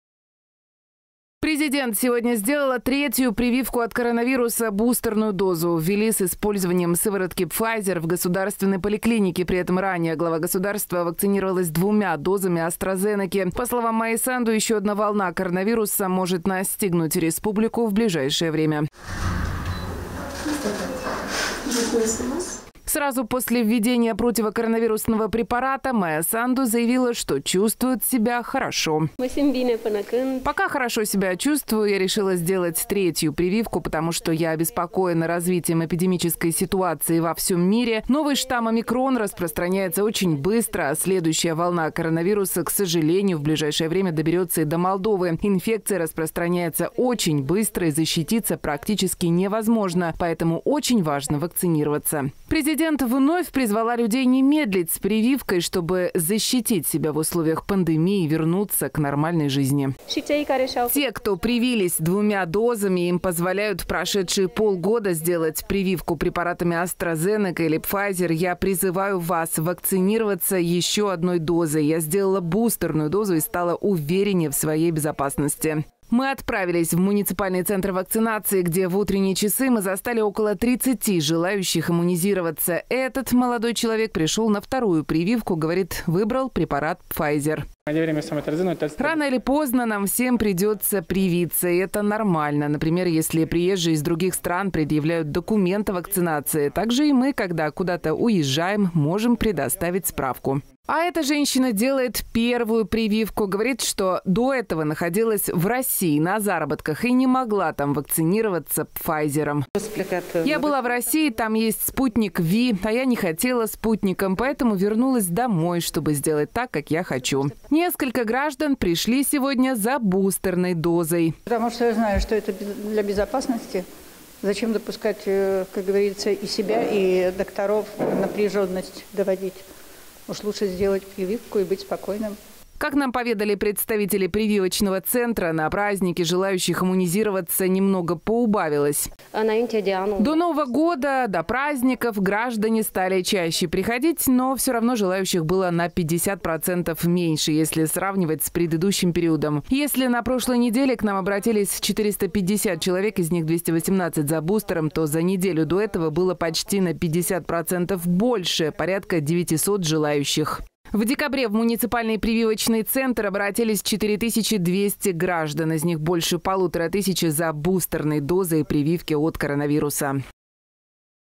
Майя Санду сделала третью прививку от коронавируса
Глава государства сегодня сделала третью прививку от коронавируса. Бустерную дозу ввели с использованием сыворотки Пфайзер в Государственной поликлинике. При этом ранее президент вакцинировалась двумя дозами Астразенеки. По словам Майи Санду, еще одна волна коронавируса может настигнуть республику в ближайшее время.